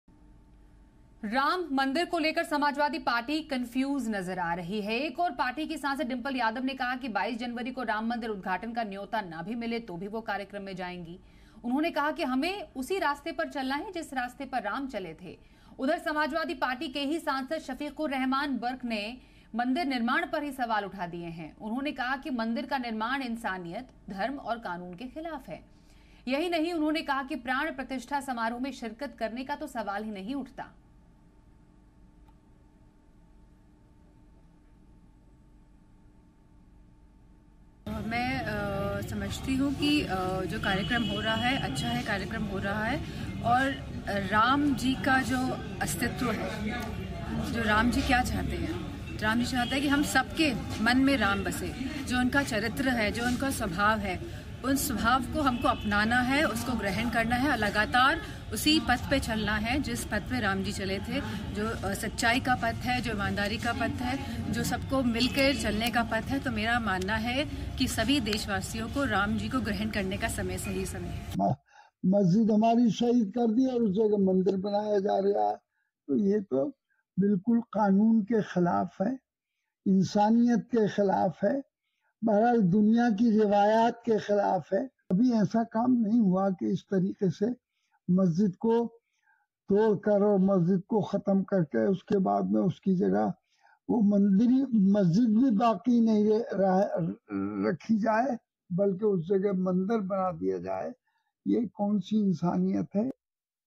Ram Mandir Inauguration: Dimple Yadav ने कहा- नहीं मिला निमंत्रण तो भी जाएंगे Ram Mandir
राम मंदिर निर्माण पर समाजवादी पार्टी में अलग अलग बयानबाजी

एक तरफ जहां डिंपल यादव राम मंदिर उद्घाटन में जाने को तैयार बैठी है वही,  दूसरी तरफ शफीकुर्रहमान बर्क ने मंदिर निर्माण पर सवाल उठाया...